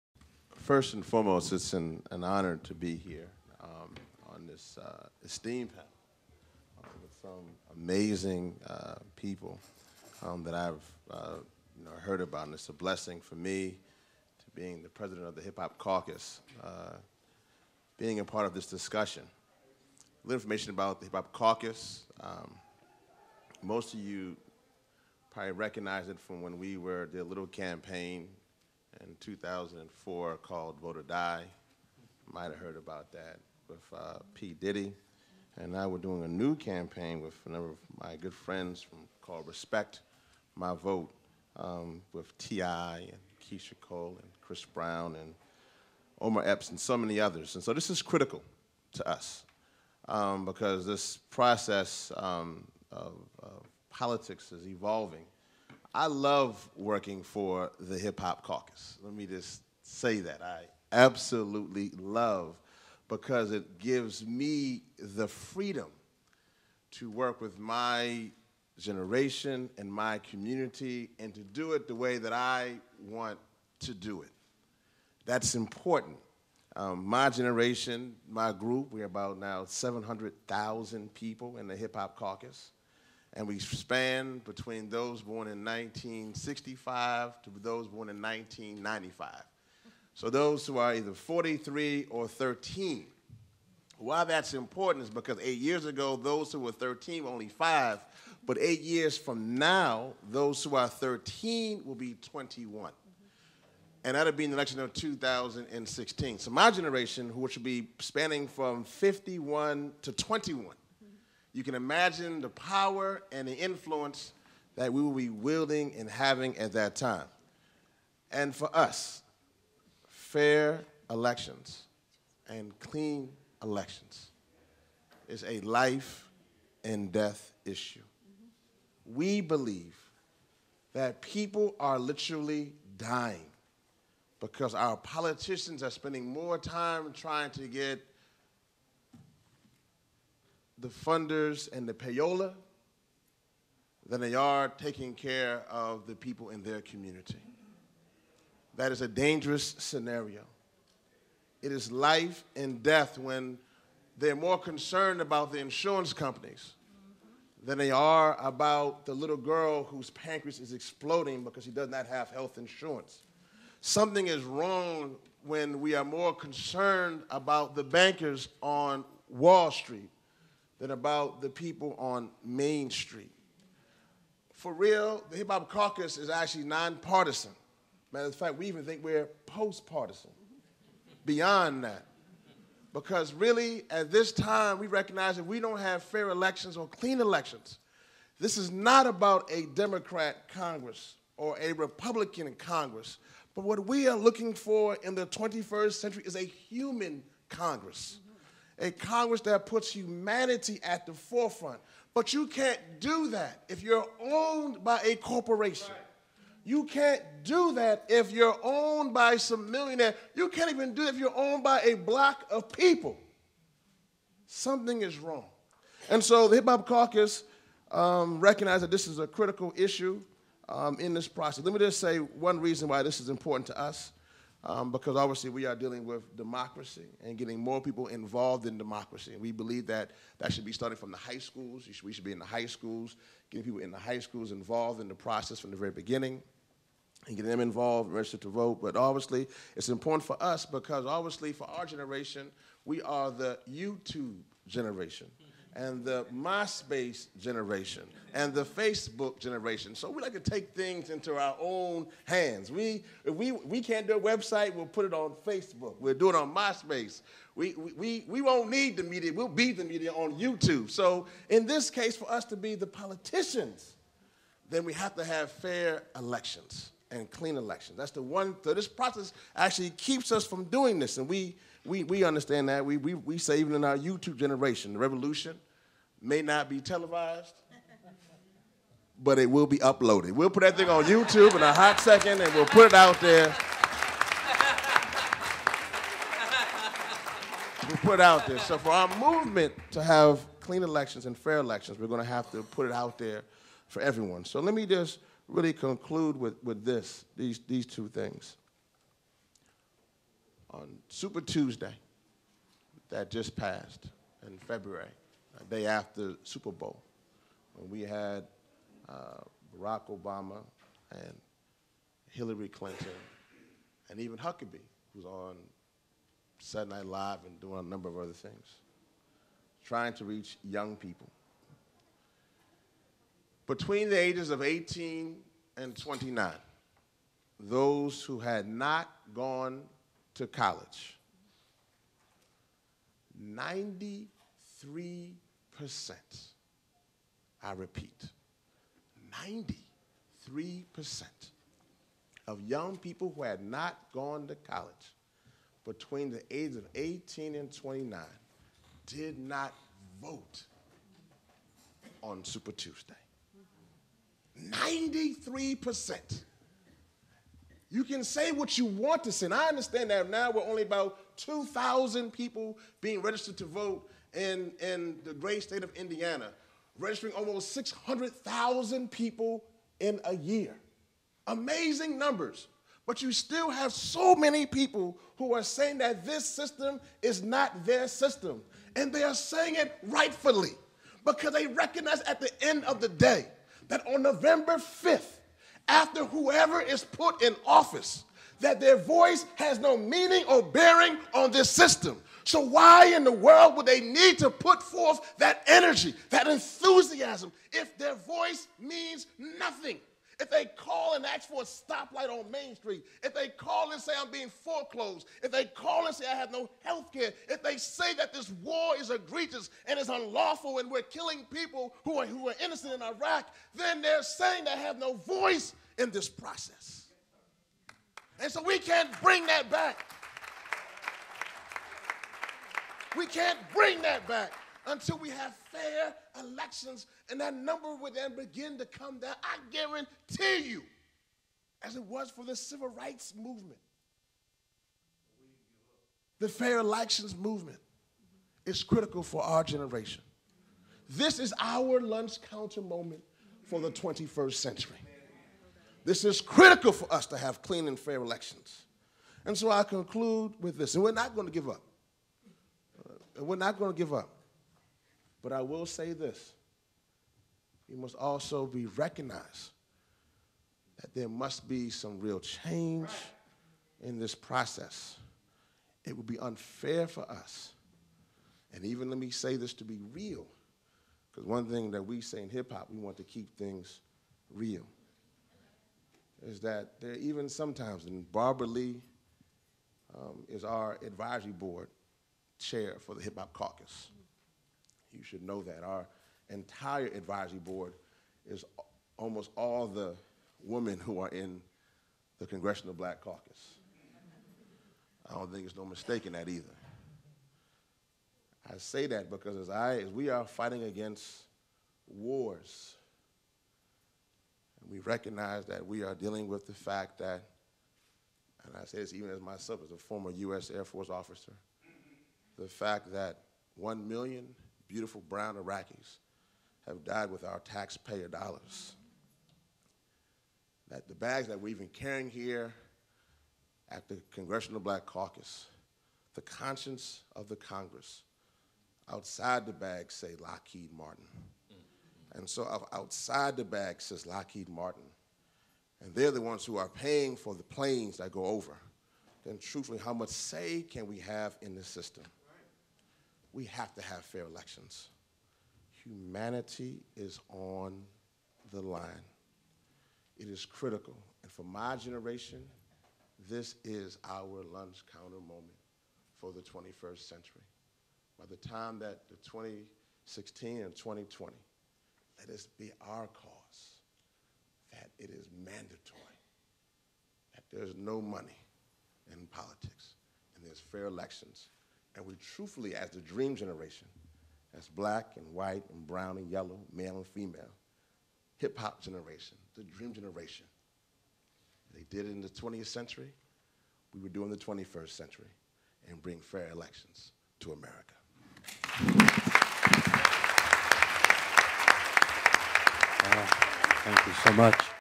Rev. Yearwood at Congressional Black Caucus Foundation
Rev. Lennox Yearwood, Jr. speaking at the Fair Elections panel on September 25, 2008 at the Congressional Black Caucus Foundation conference.